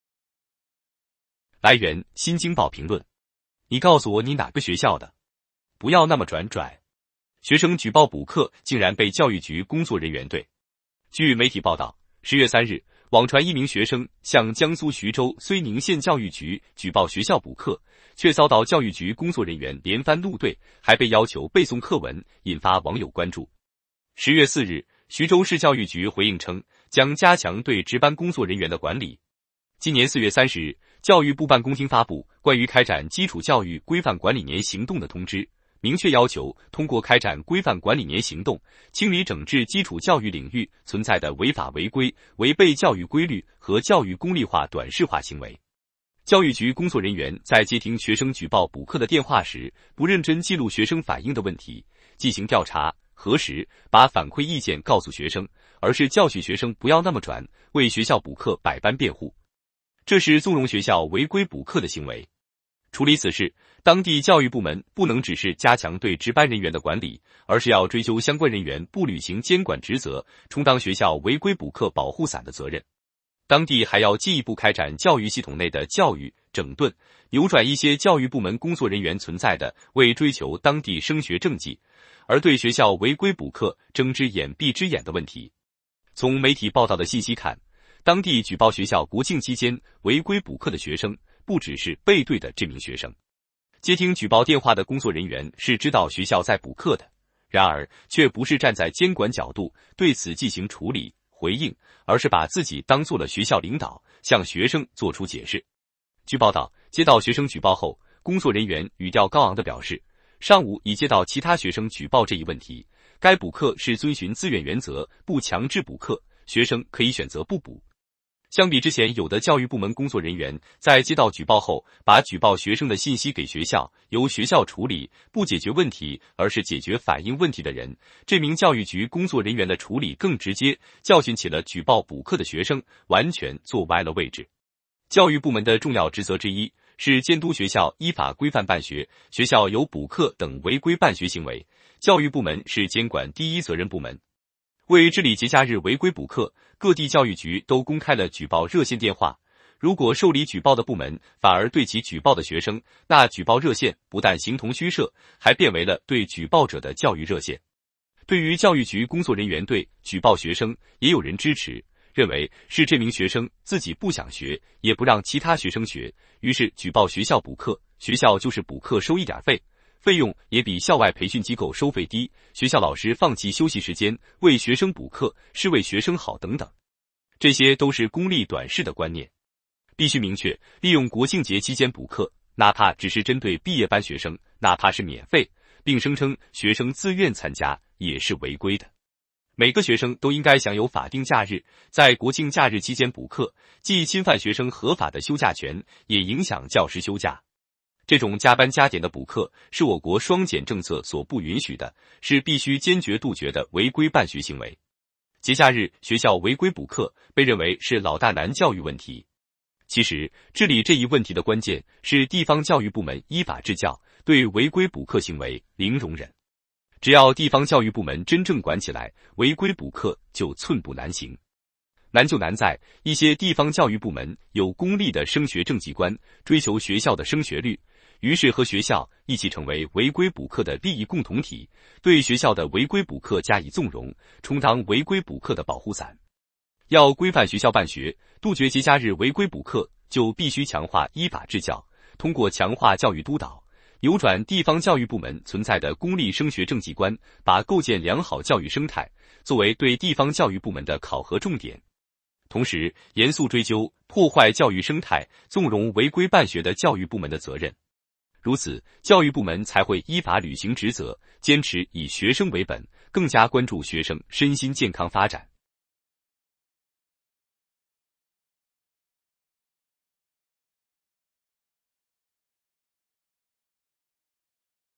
教训举报学校补课学生， 教育局工作人员站哪边？
来源: 新京报评论
“你告诉我你哪个学校的！不要那么转（zhuǎi）！”学生举报补课竟然被教育局工作人员怼。据媒体报道，10月3日，网传一名学生向江苏徐州睢宁县教育局举报学校补课，却遭到教育局工作人员连番怒怼，还被要求背诵课文，引发网友关注。